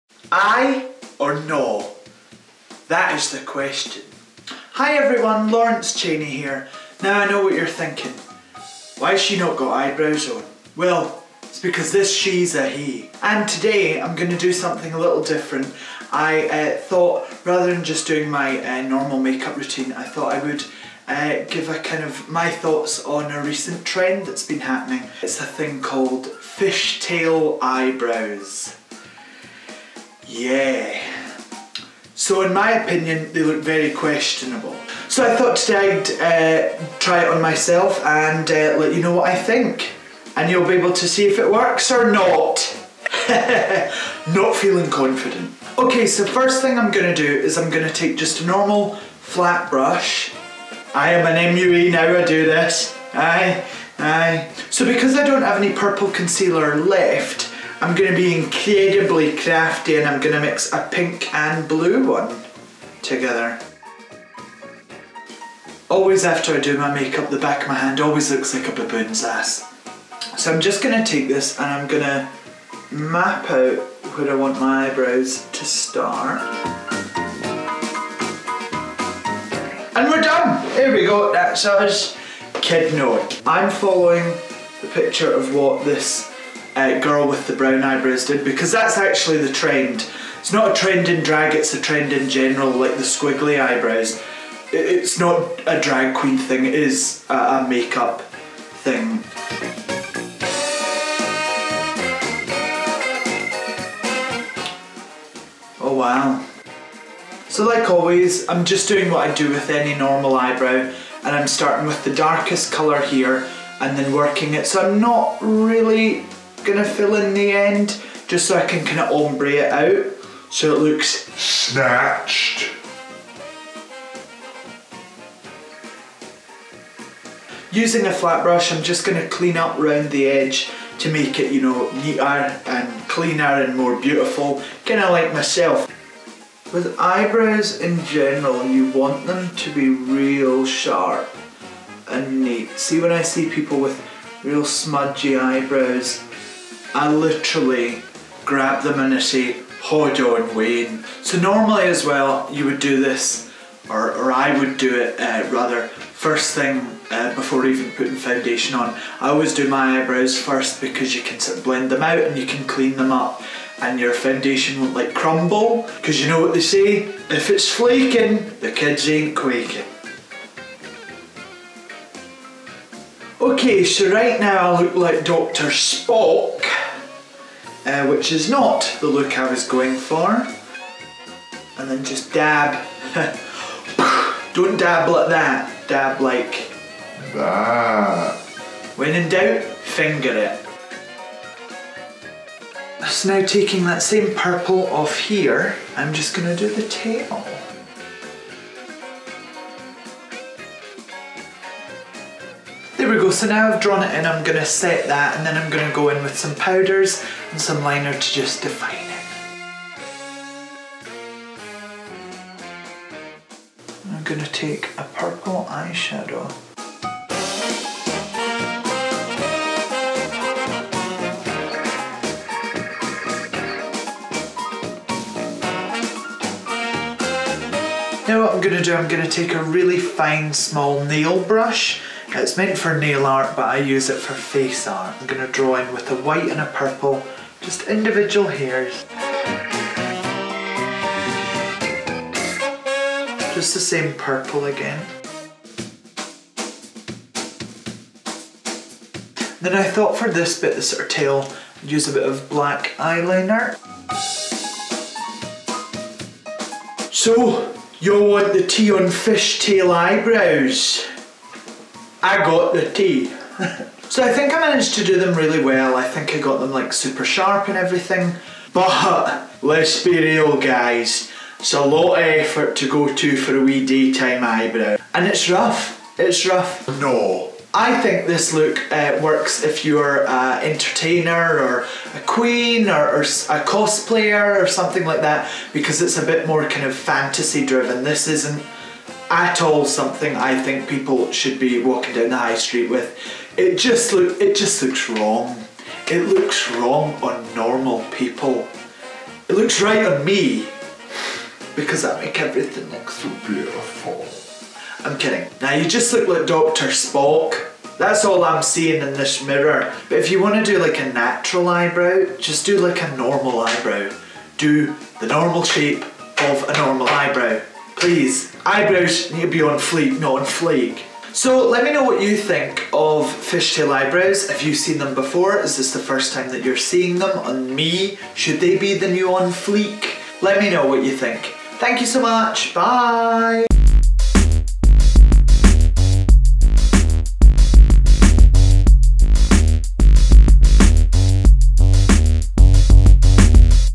Lawrence Chaney Tries Fishtail Eyebrows
Glasgow drag queen Lawrence Chaney is testing a new eyebrow trend, fishtail eyebrows. Watch as he goes from nae eyebrows, to Spock, to fishtail glamour.

What's the verdict? Fishtail eyebrows, aye or naw?